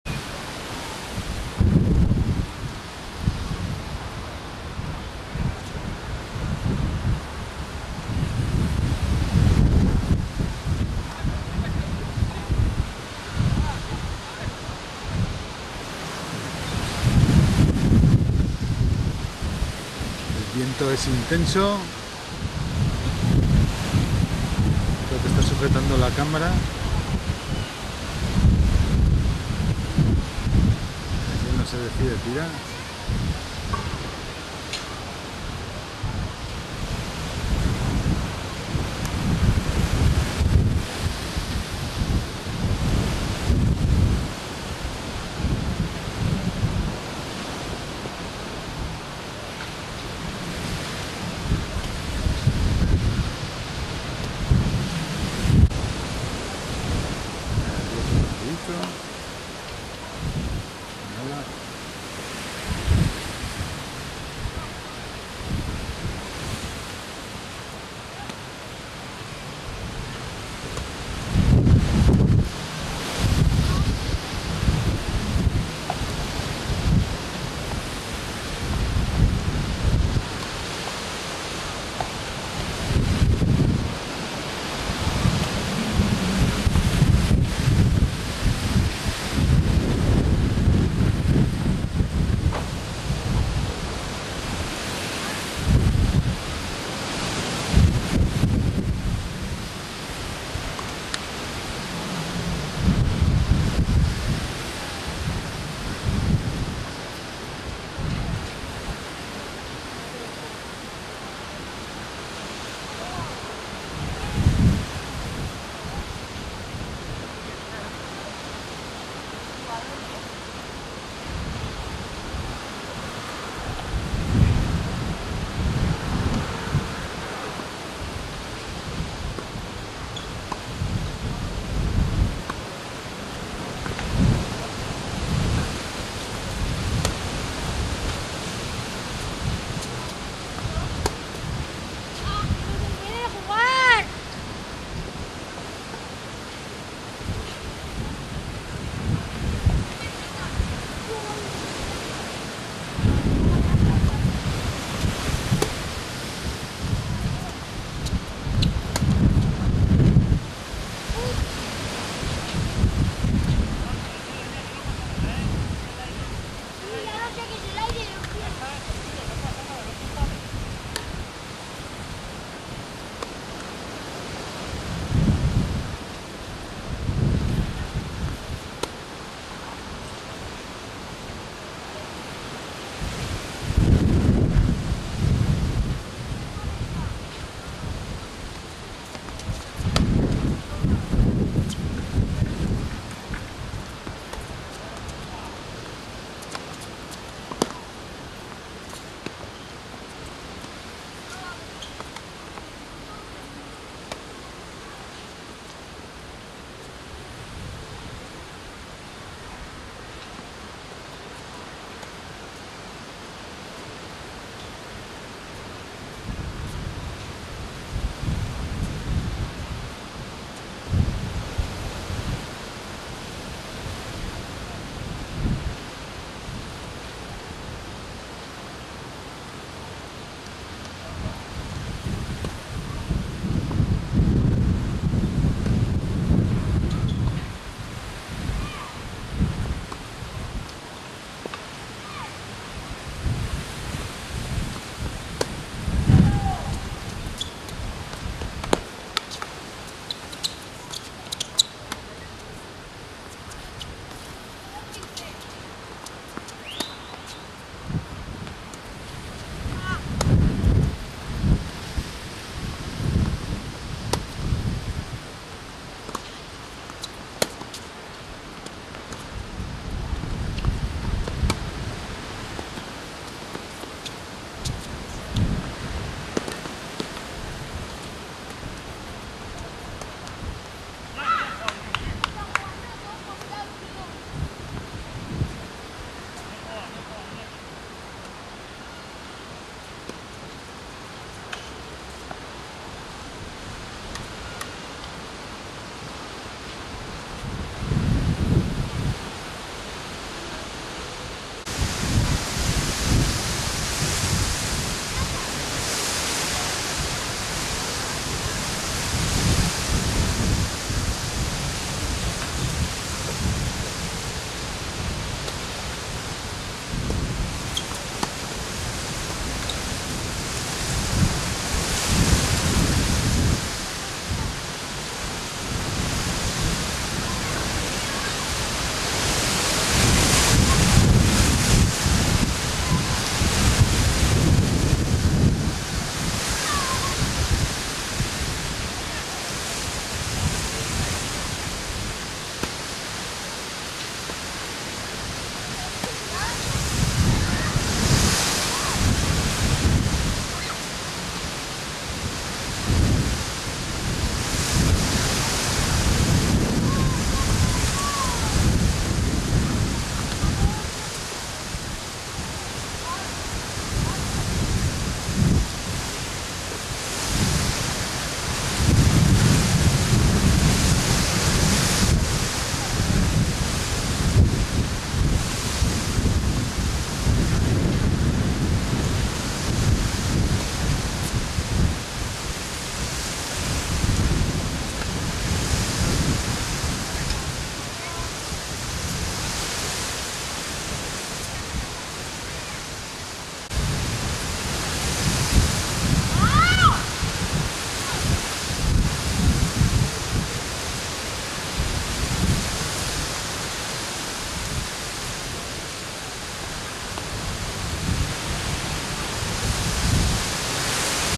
Tenis alevín en Cadreita, 4º torneo, 27 octubre 2012
Este es un vídeo cortito de 6 minutos del torneo de tenis alevín que se celebró el día 27 de octubre. El fuerte viento movía la pelota a su antojo y hacía difícil jugar los partidos de tenis.